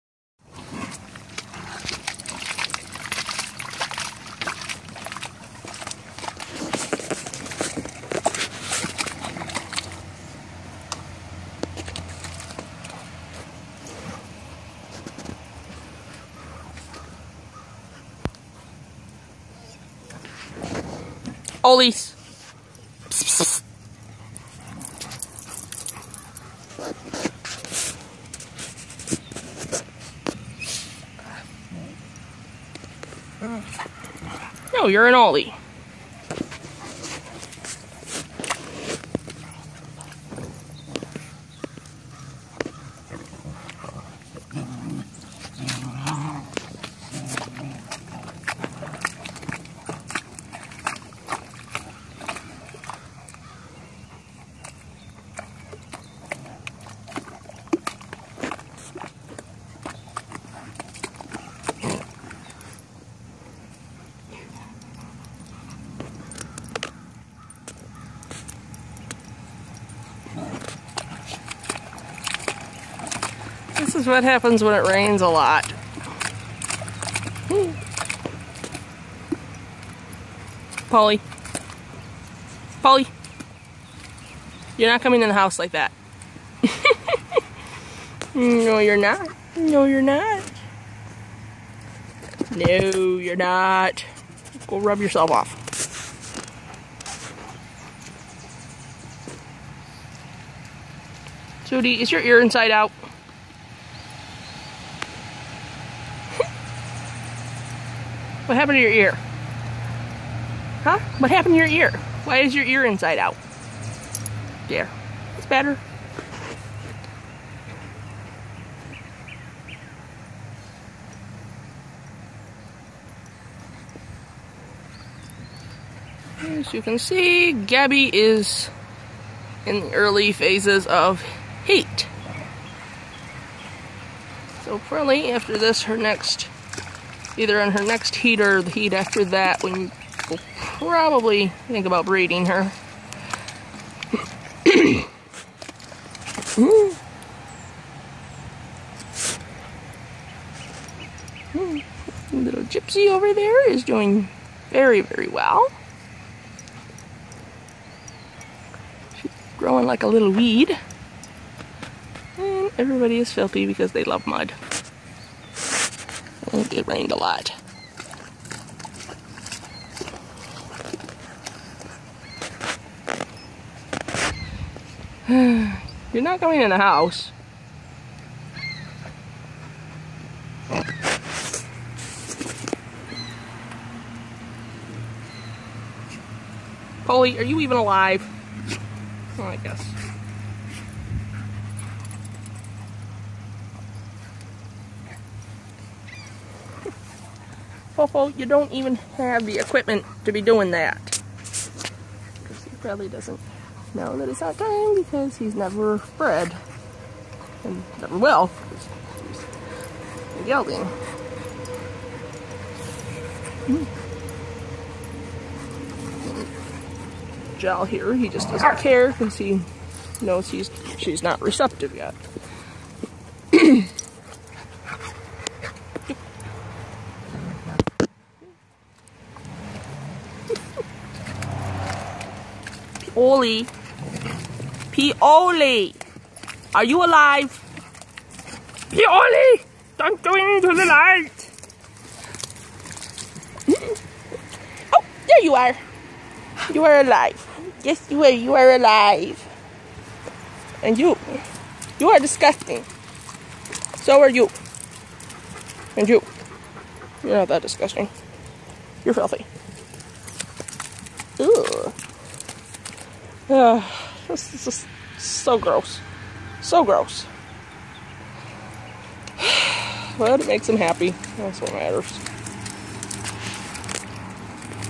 You're not coming in the house like that...
It's been a hectic couple of weeks. Gabby's in heat for the first time, Jypsy is doing well, one of the beagles has been rehomed after killing a pullet and wounding another, I put up some new fencing, sold some chickens, got a second batch of pullet chicks, and moved the older ones outside, among other things. Just another day in Paradise.